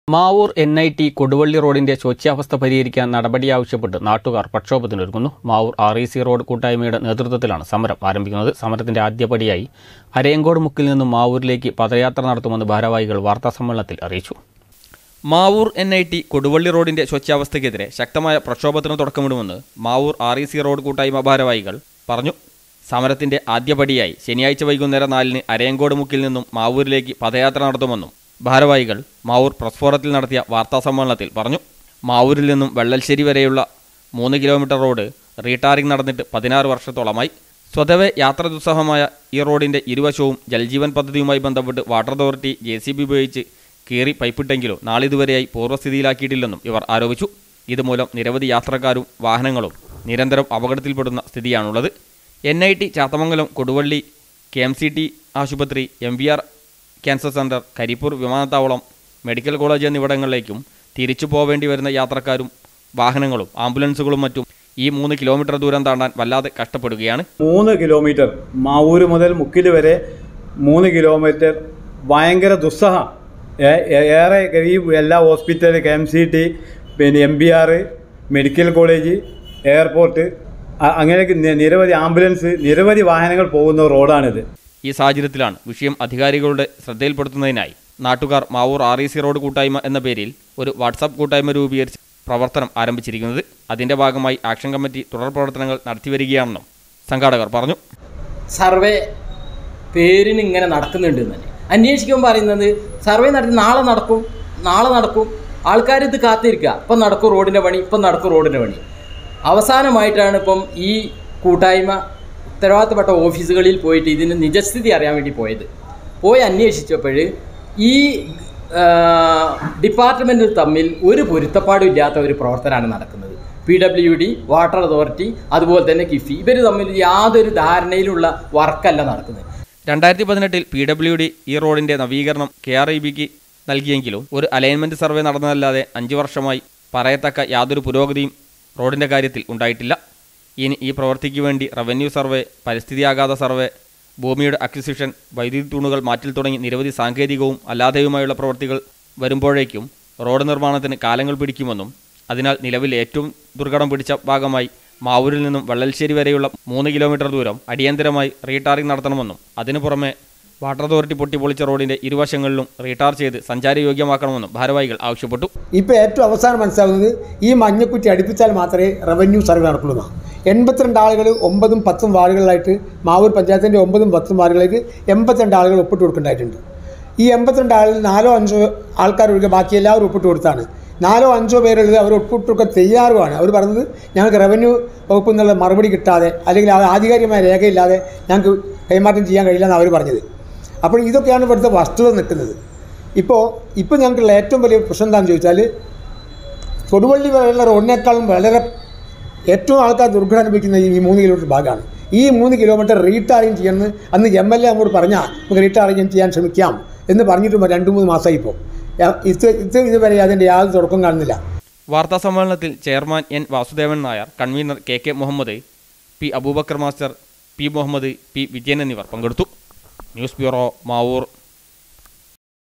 മാവൂർ എൻ -ഐ- ടി കൊടുവള്ളി റോഡിൻ്റെ ശോച്യാവസ്ഥ പരിഹരിക്കാൻ നടപടിയാവശ്യപ്പെട്ട് നാട്ടുകാർ
മാവൂർ എൻ -ഐ- ടി കൊടുവള്ളി റോഡിൻ്റെ ശോച്യാവസ്ഥ പരിഹരിക്കാൻ നടപടിയാവശ്യപ്പെട്ട് നാട്ടുകാർ പ്രക്ഷോഭത്തിനൊരുങ്ങുന്നു.